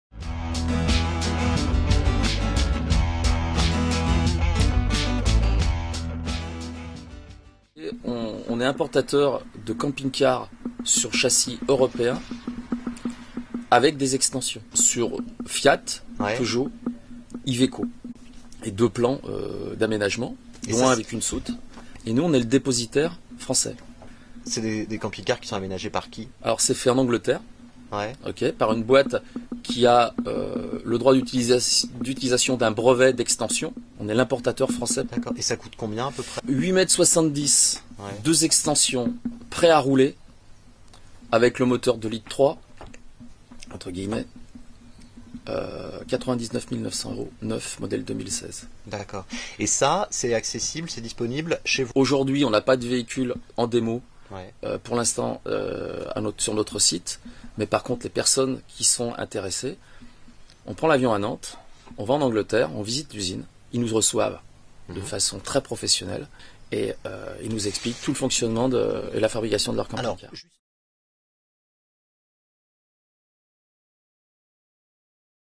Moto-Trek: les camping-cars anglais à slide-out arrivent en France
Eric Terrasson commercialise sur son site internet les camping-cars de la marque anglaise Moto-Trek. Il répond aux questions du Monde du Camping-Car.